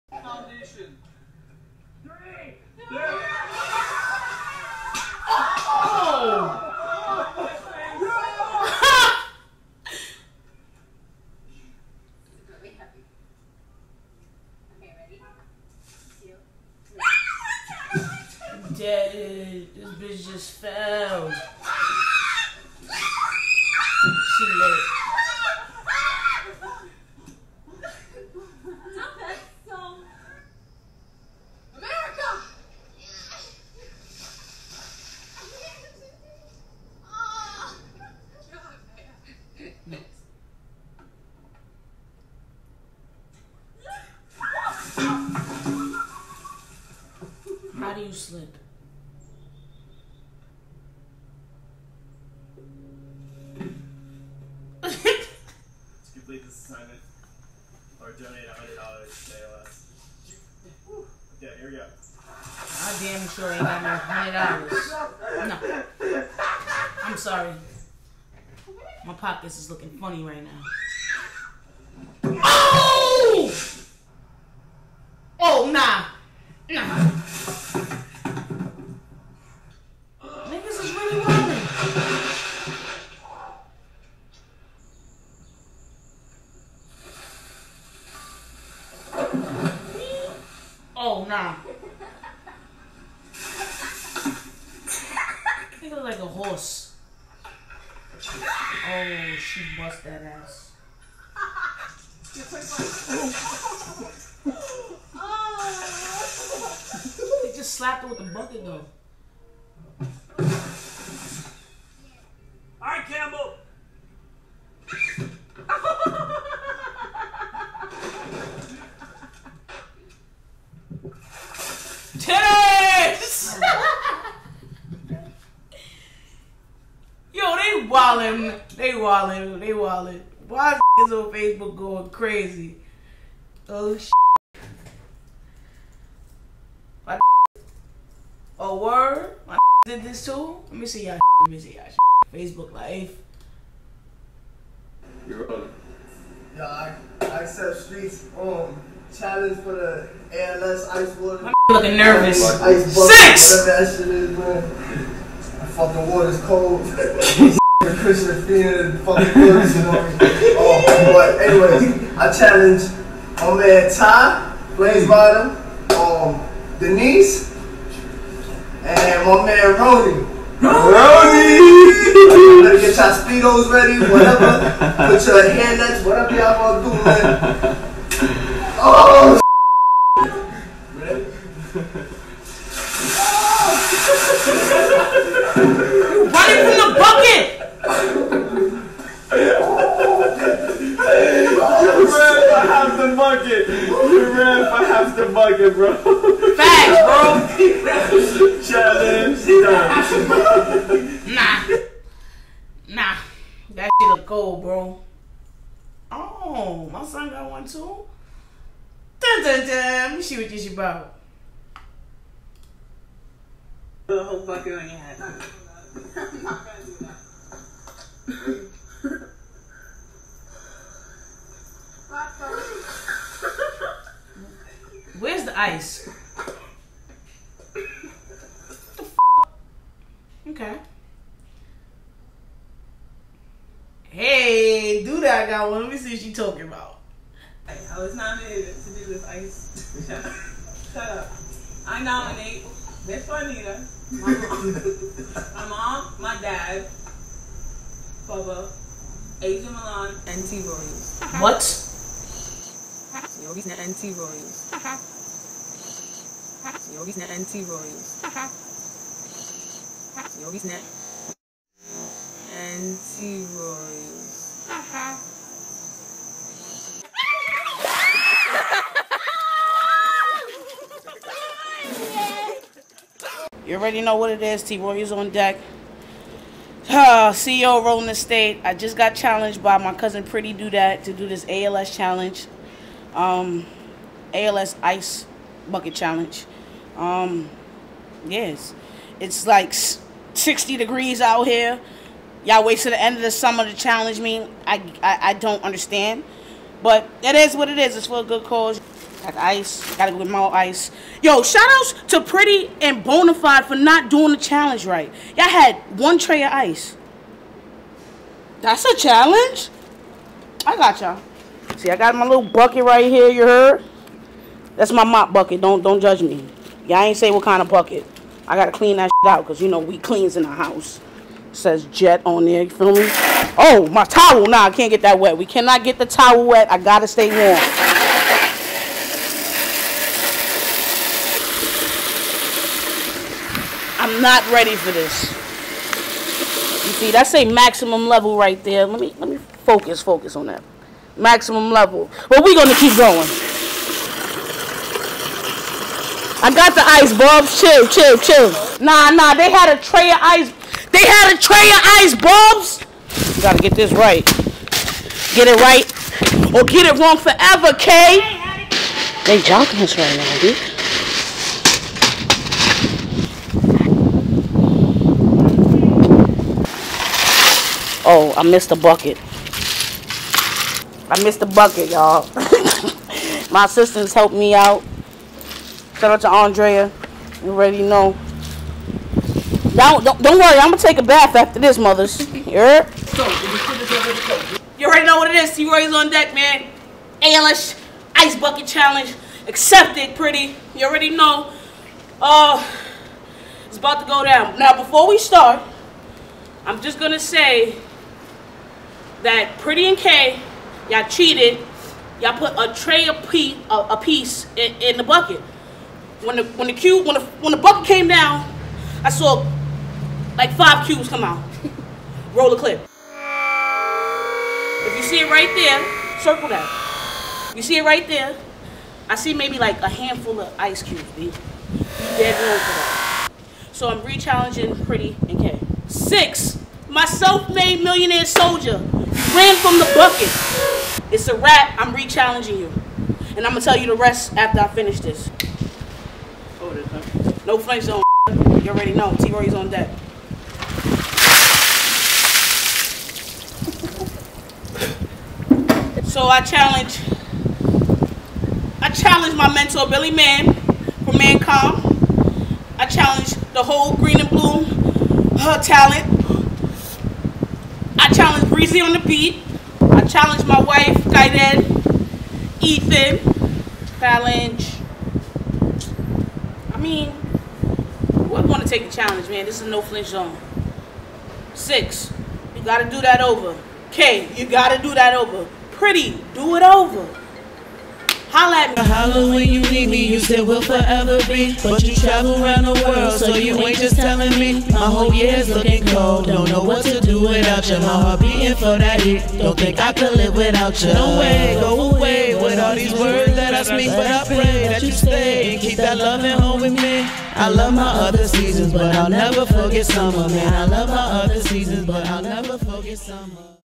T-Royalz Ice Bucket Challenge
Pretty DoDat Challenges T-Royalz to the ALS Ice Bucket Challenge. Watch The Chaos.